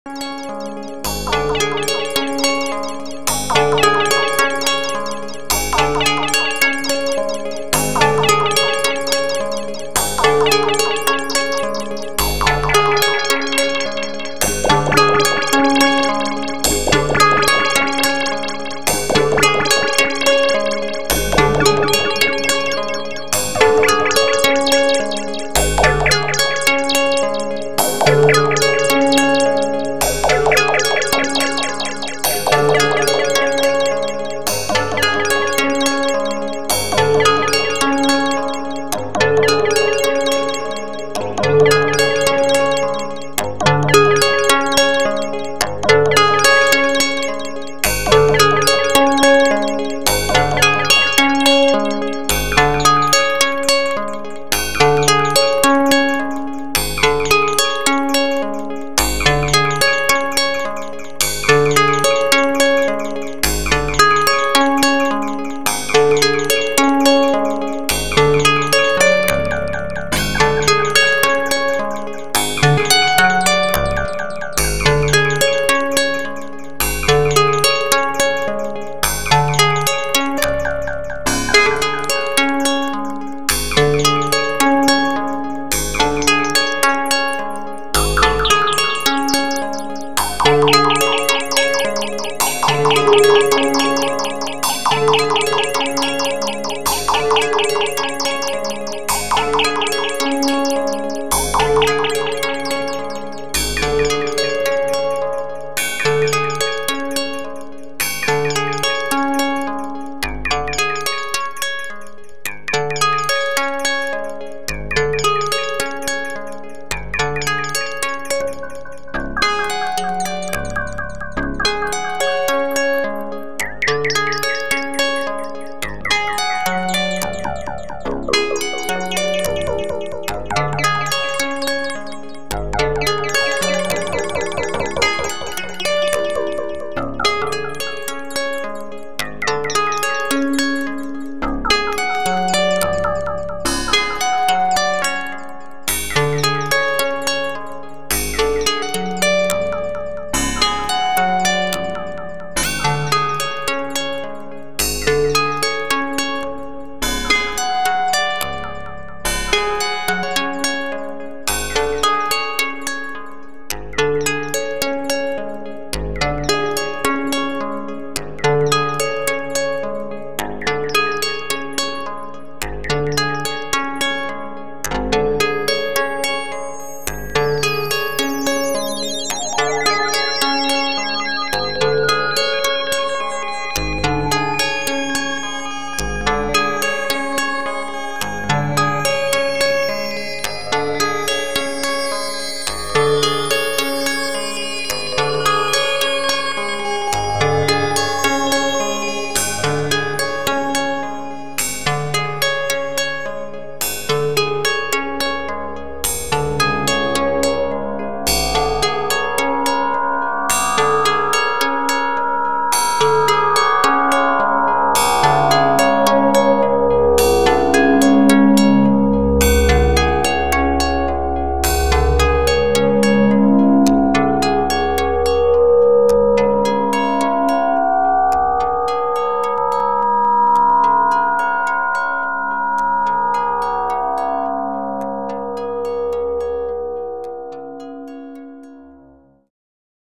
Modular Synthesizer // buddhist minimalist is an alien
Fooling around with a simple patch ...

VCV Rack
Novation Launch Control XL
Eurorack Modular: Rings & Plaits & Plonk